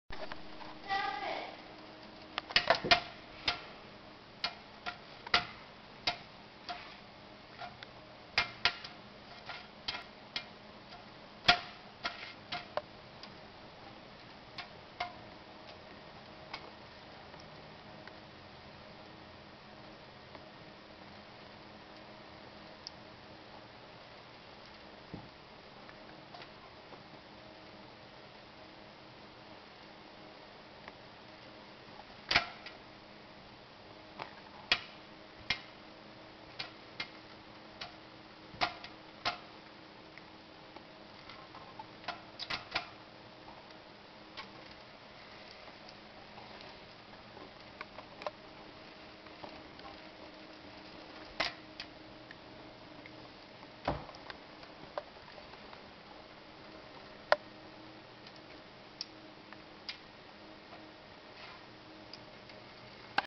My white cat "Smudge" loves water when he can catch it
Smudge has a bowl of water on the floor but he insists on catching his own. At least I won't need to wash him.